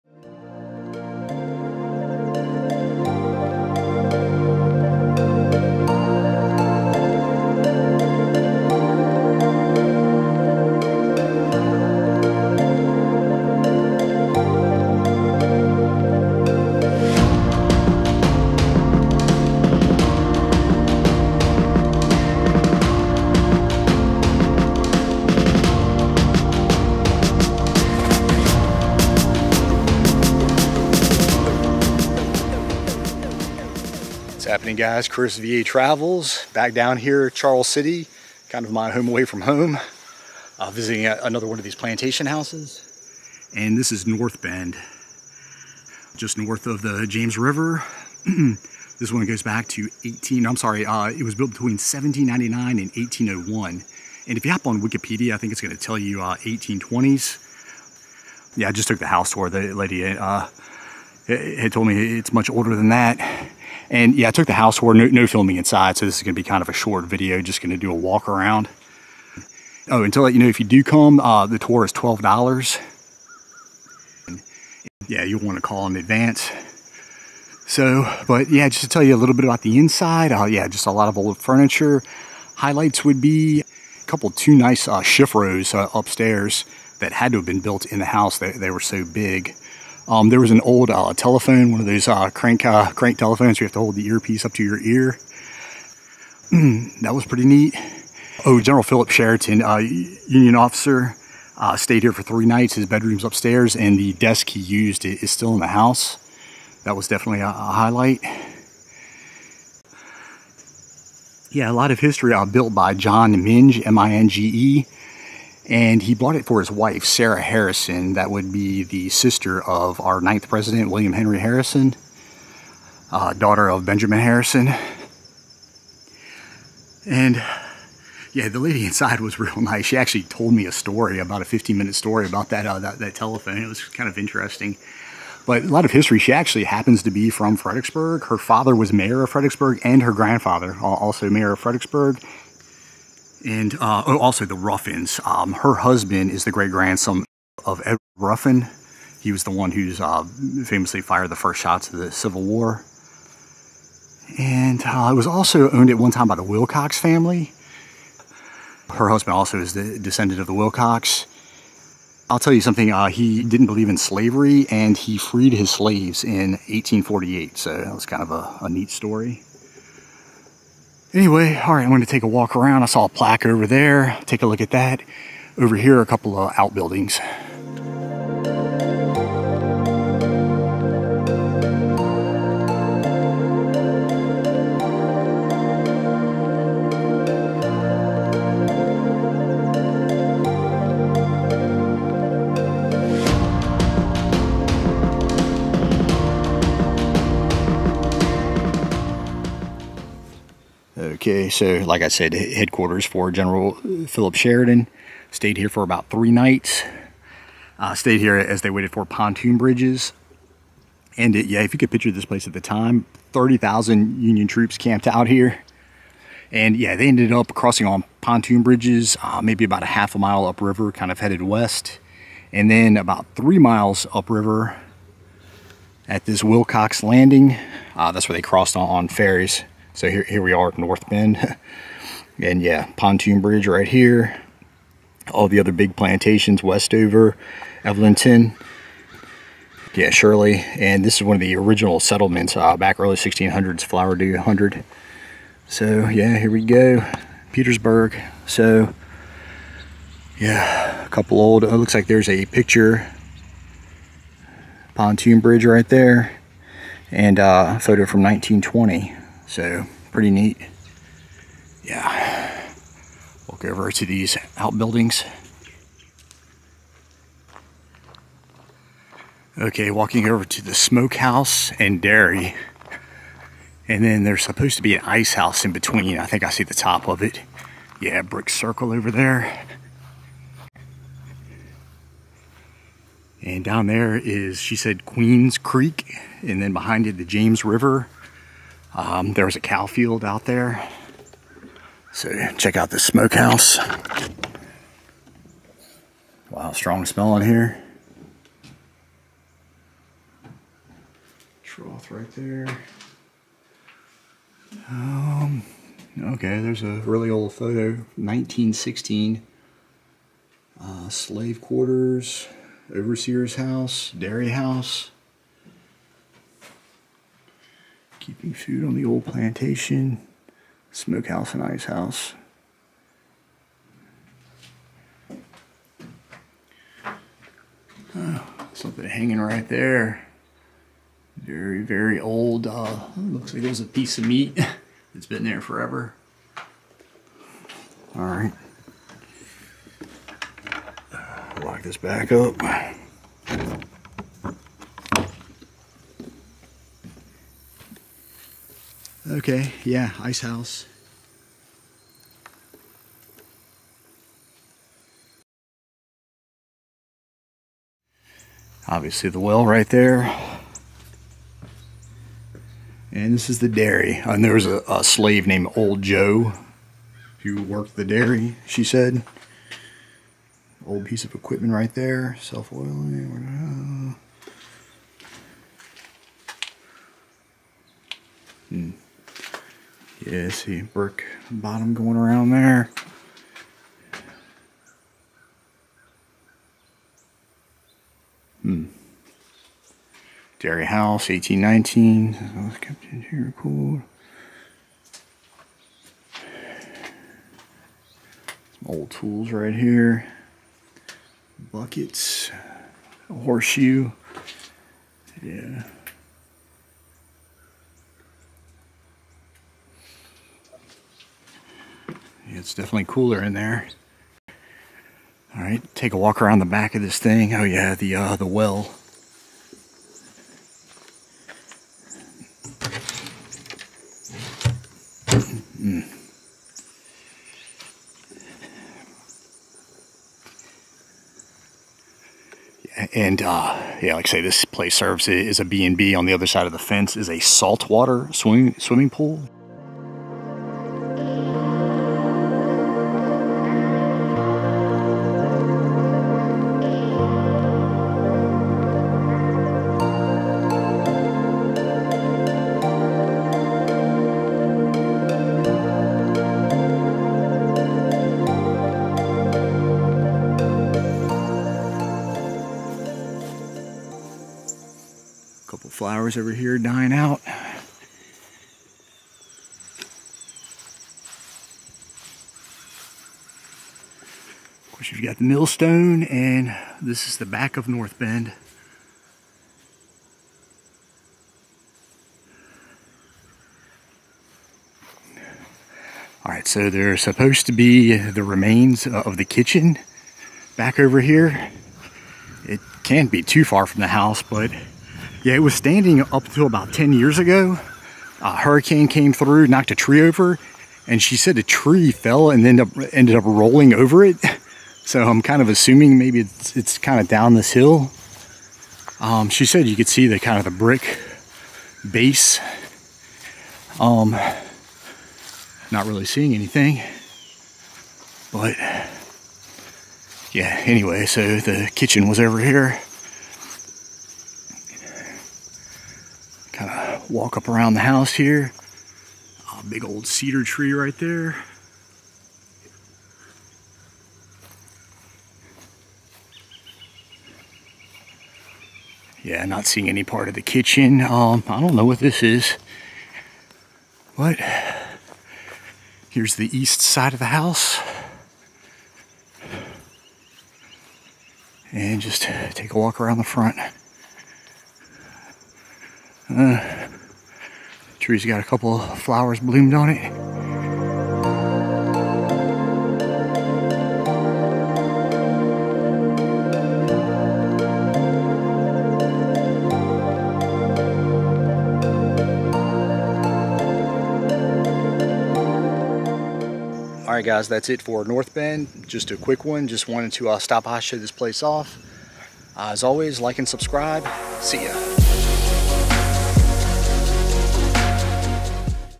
NORTH BEND PLANTATION (Charles City County, VA)
Historic home off the James River in Charles City County, Virginia. Built my John Minge in 1801 for his wife Sarah Harrison (sister of President William Henry Harrison).

Used as headquarters for General Sheridan during the Overland Campain.